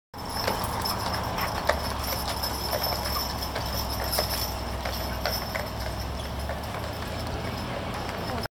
Savannah Horse Cart
A tour in Savannah GA